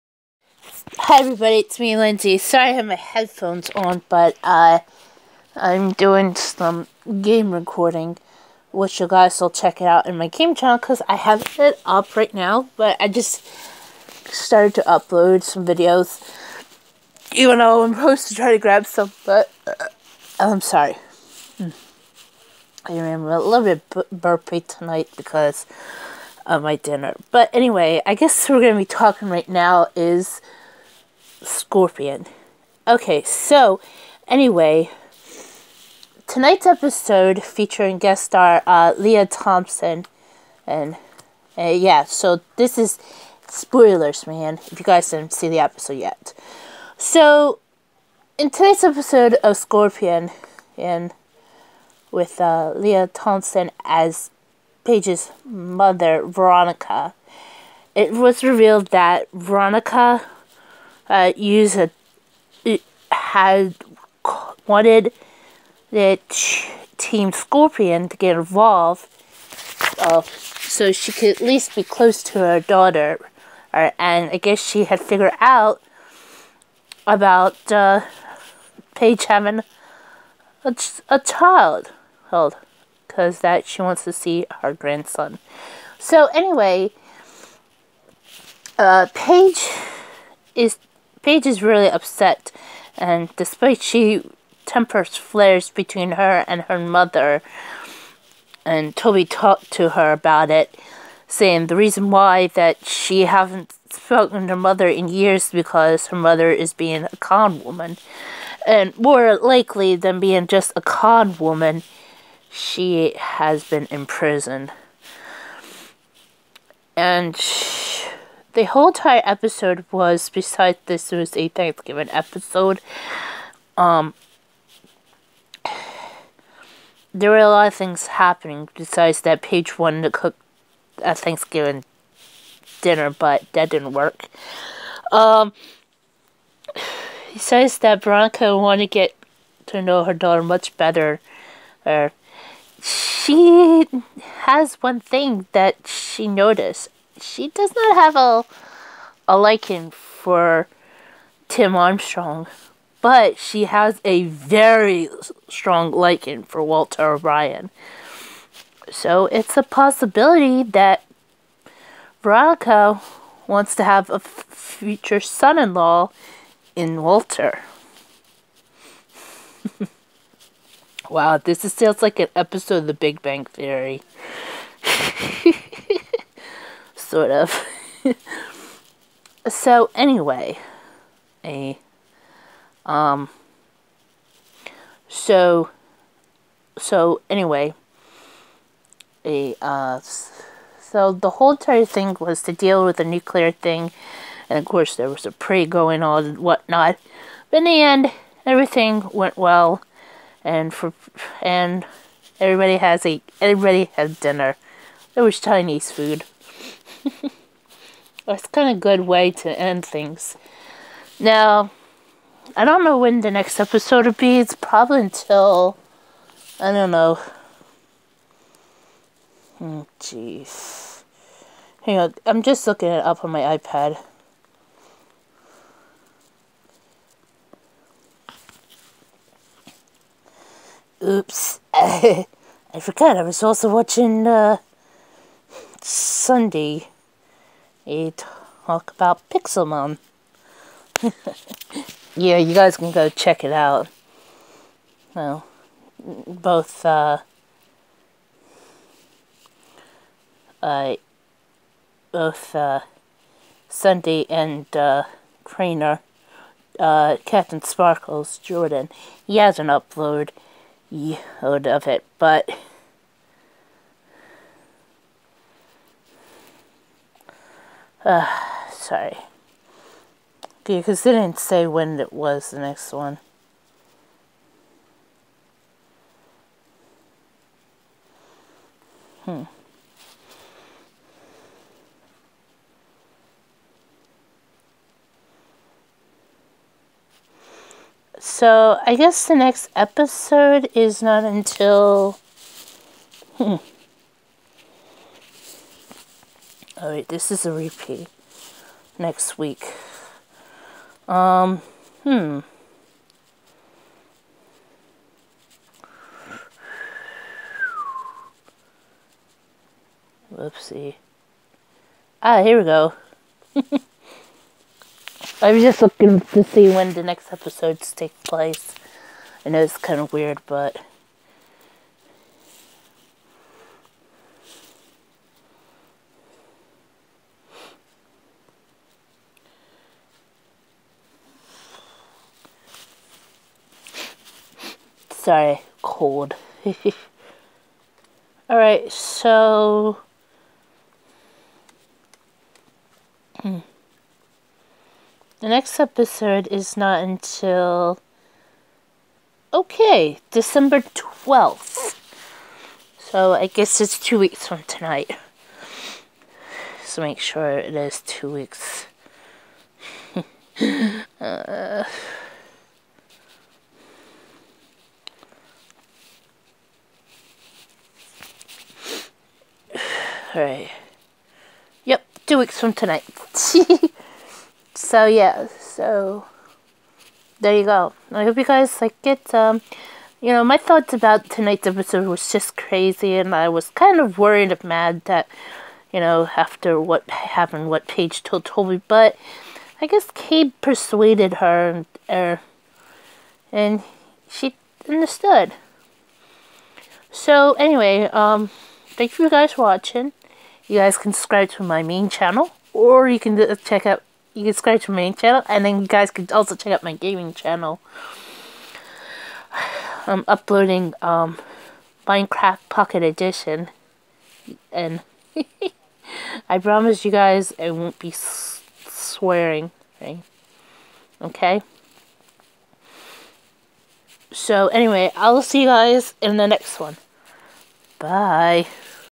Lindsie Starr's tv review: Scorpion
My thoughts about tonight's episode of Scorpion.